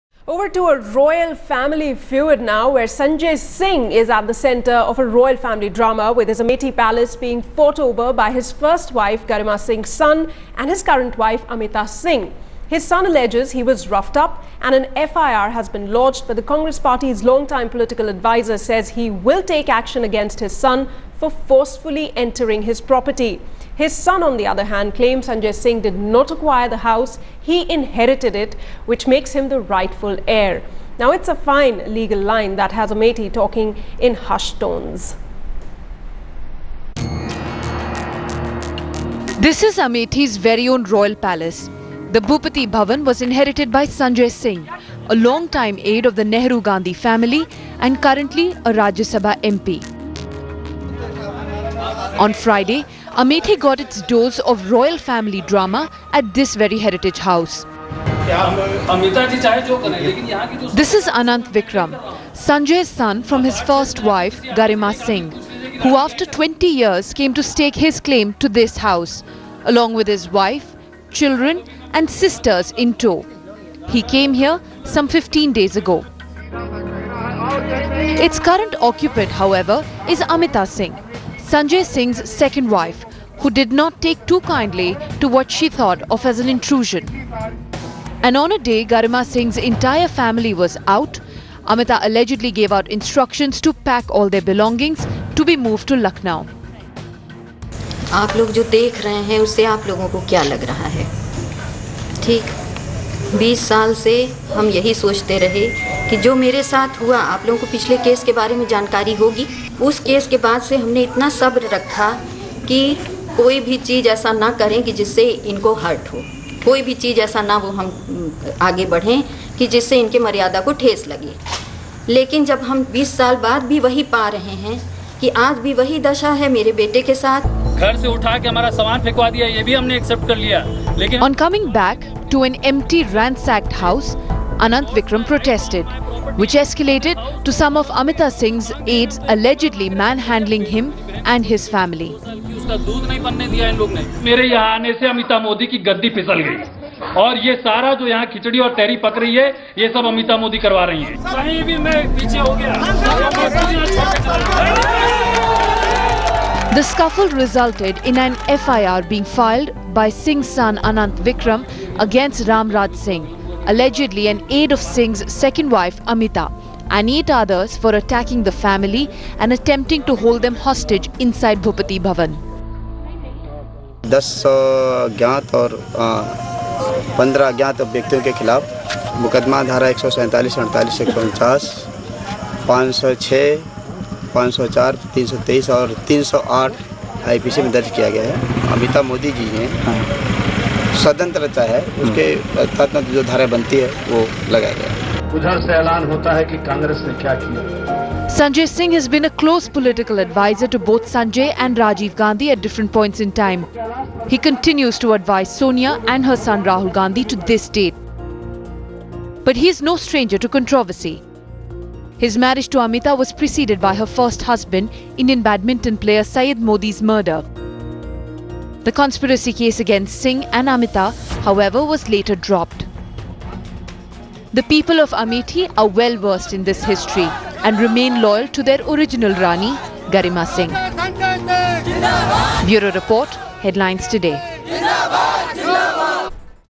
Congress confidant Sanjay Singh's son files FIR about inheritance
This is an age old story. First wife's children claim their father's property while the second wife tries to hold on to it, even though inheritance rights in this country are pretty clear. But, when the property is a royal one – how does normal-man legality apply to it? Listen to this clip to understand the Amethi Bhupathi Bhavan inheritance controversy.